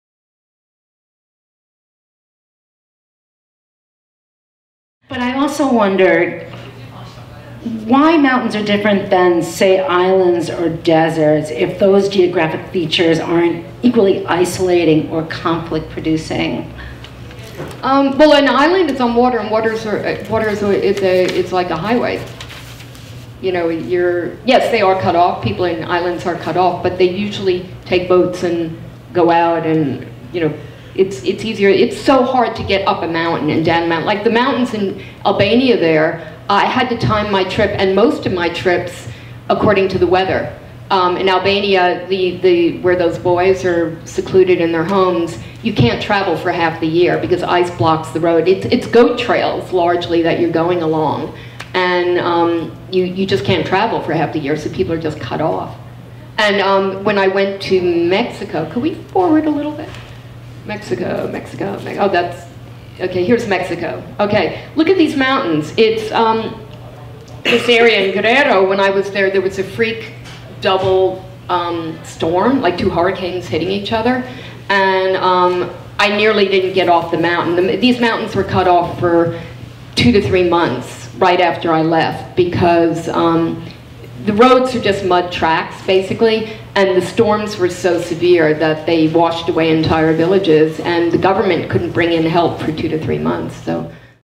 Book Night: No Friends but the Mountains - Judith Matloff (Mountains Are Like Islands)
On March 22, the OPC and the Columbia Graduate School of Journalism hosted a book night to discuss No Friends but the Mountains - Dispatches from the World's Violent Highlands with author Judith Matloff. Her book, which took eight years to report and 72,000 miles of travel, explores why mountainous regions are disproportionate share of the world's conflicts.

Matloff is adjunct associate professor of journalism at the Columbia Graduate School of Journalism.

Mary D'Ambrosio, a journalist who specializes in writing about international affairs who is also an associate professor at Rutgers University, moderated the discussion.